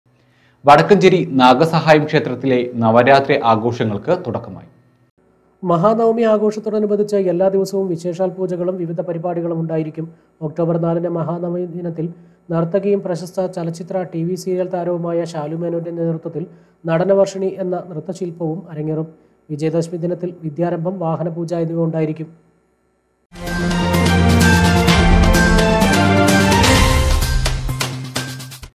വടക്കഞ്ചേരി നാഗ സഹായം ക്ഷേത്രത്തിൽ നവരാത്രി ആഘോഷങ്ങൾക്ക് തുടക്കമായി
StarNet Online

വാർത്തകൾ   അറിയിക്കാൻ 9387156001, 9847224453, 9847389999 എന്ന നമ്പറിൽ വിളിക്കുക

StarNet Online